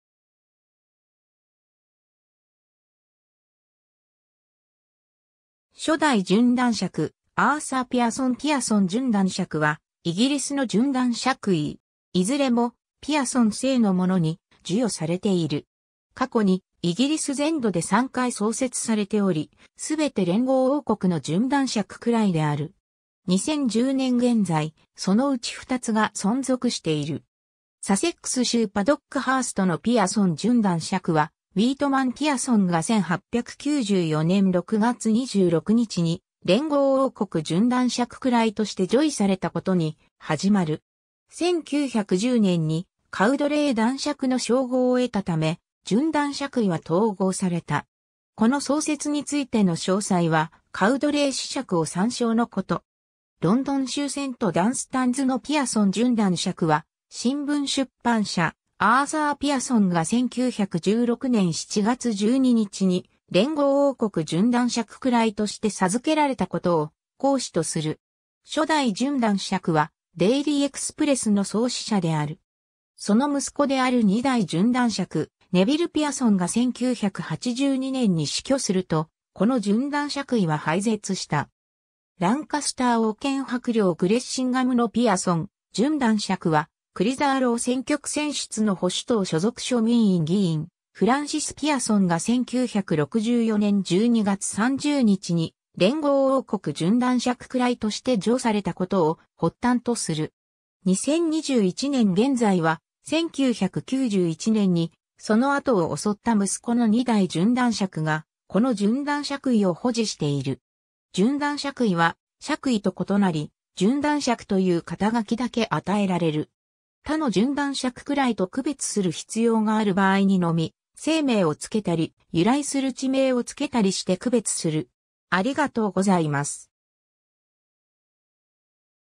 ピアソン準男爵
初代準男爵アーサー・ピアソン ピアソン準男爵（ピアソンじゅんだんしゃく、Pearson baronets）は、イギリスの準男爵位。
いずれも「ピアソン」姓の者に授与されている。
過去にイギリス全土で3回創設されており、全て連合王国の準男爵位である。
2010年現在、そのうち2つが存続している。
 サセックス州パドックハーストのピアソン準男爵(Pearson Baronetcy, of Paddockhurst, in the County of Sussex)は、ウィートマン・ピアソンが1894年6月26日に連合王国準男爵位として叙位されたことに始まる。
1910年にカウドレー男爵の称号を得たため、準男爵位は統合された。
この創設についての詳細は、カウドレー子爵を参照のこと。
 ロンドン州セント・ダンスタンズのピアソン準男爵(Pearson Baronetcy, of St. Dunstan's in the County of London)は、新聞出版者アーサー・ピアソンが1916年7月12日に連合王国準男爵位として授けられたことを嚆矢とする。
初代準男爵は『デイリー・エクスプレス』の創始者である。
その息子である2代準男爵ネヴィル・ピアソンが1982年に死去すると、この準男爵位は廃絶した。
 ランカスター王権伯領グレッシンガムのピアソン準男爵(Pearson Baronetcy, of Gressingham in the County Palatine of Lancaster)は、クリザーロー選挙区選出の保守党所属庶民院議員フランシス・ピアソンが1964年12月30日に連合王国準男爵位として叙されたことを発端とする。
2021年現在は、1991年にその後を襲った息子の2代準男爵がこの準男爵位を保持している。
 ^ 準男爵位は爵位と異なり、準男爵という肩書だけ与えられる（「○○準男爵」といった形では与えられない）。
他の準男爵位と区別する必要がある場合にのみ姓名を付けたり、由来する地名を付けたりして区別する